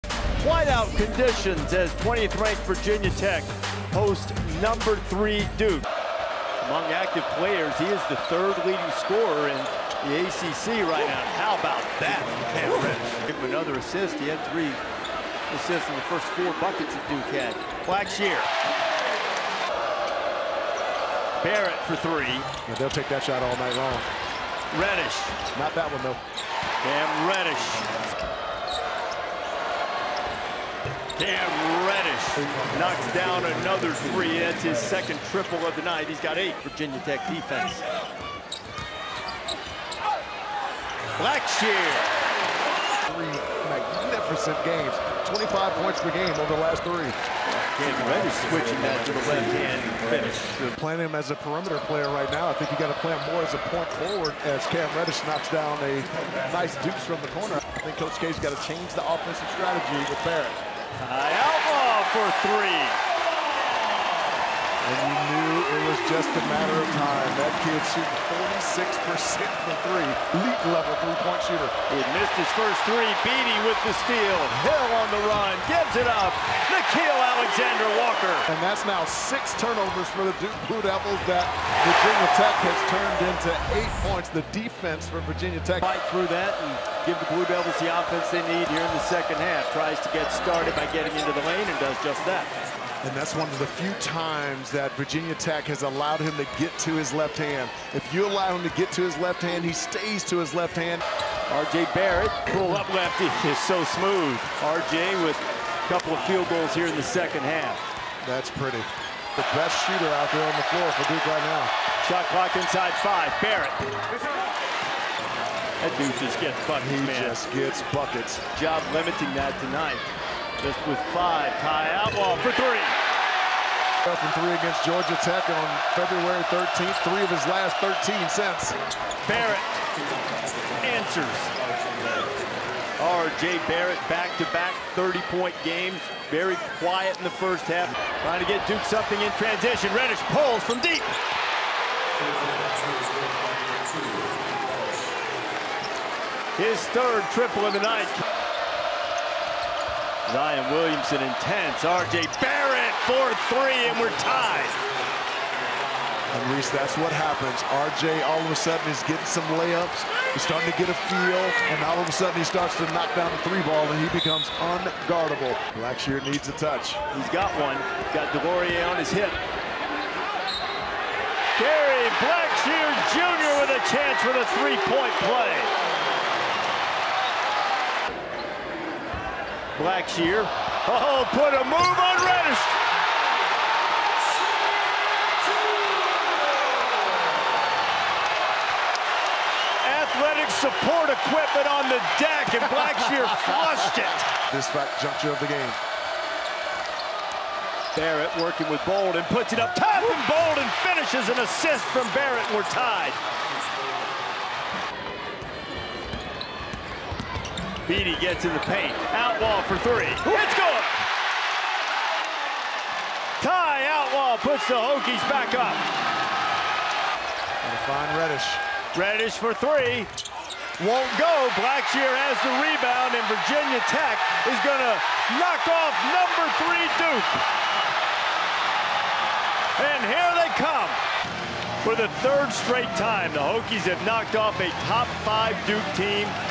With Zion Williamson out, Duke loses to Virginia Tech | College Basketball Highlights
Virginia Tech's Ty Outlaw hits a critical 3-pointer with 1:30 left and Kerry Blackshear Jr. leads all scorers with 23 points as the Hokies defeat Duke 77-72. RJ Barrett tallies 21 points and Cam Reddish adds 17 for the Blue Devils in the loss.